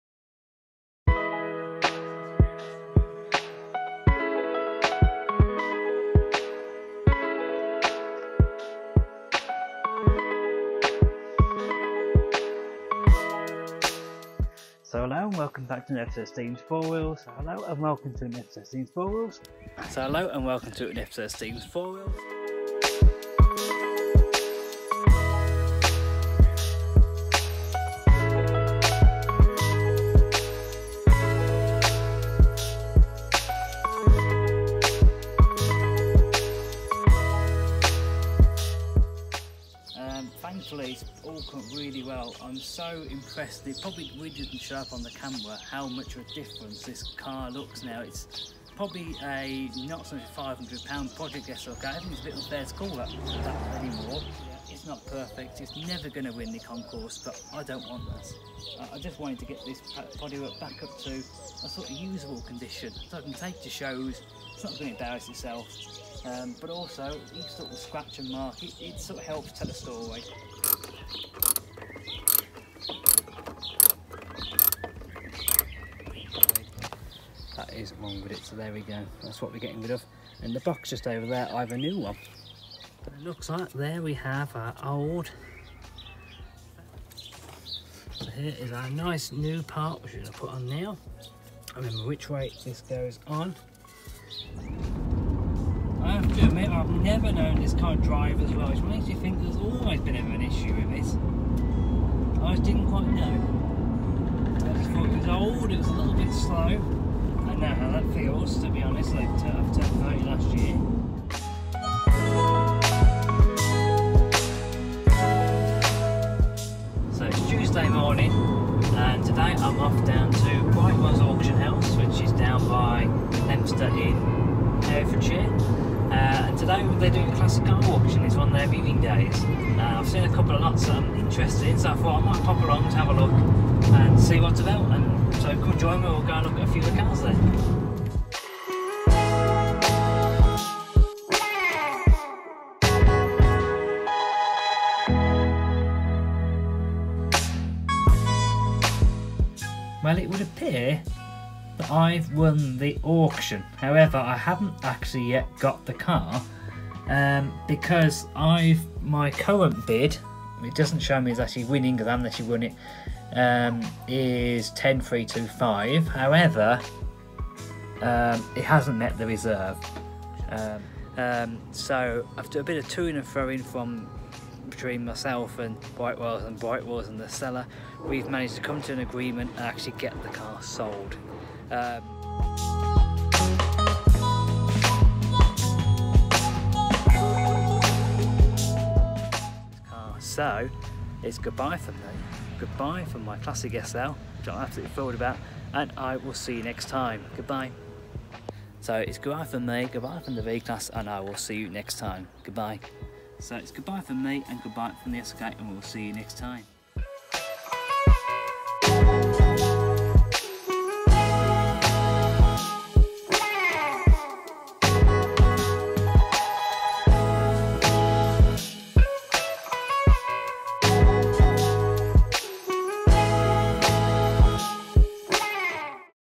What a great year 2023 was!
In this episode of Steven’s Four Wheels, we review what made 2023 such a great year!